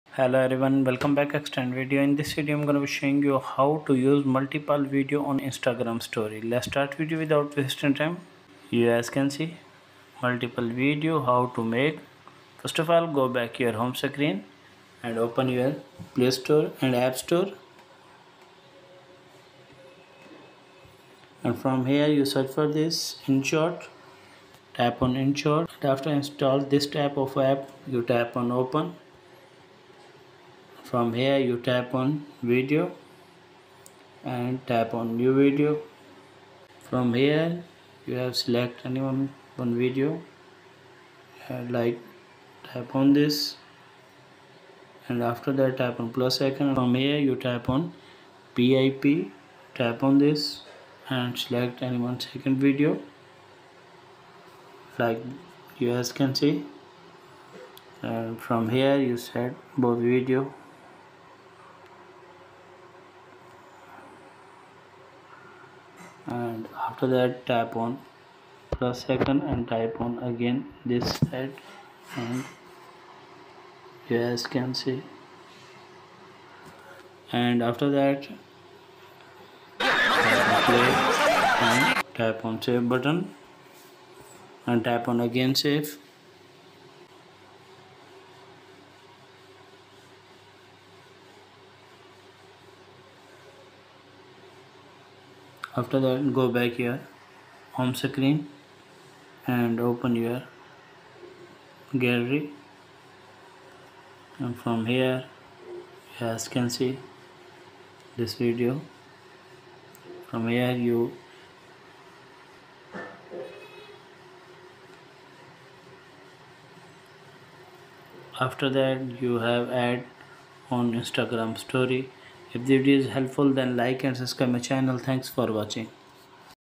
How to Use Multiple Videos On INSTAGRAM Story | How to put multiple videos on instagram story | 2023
Hi Everyone Well Come to My Channel. In this video I gonna Show you " how to use multiple videos on instagram story  " on Your iPhone and Android Phone .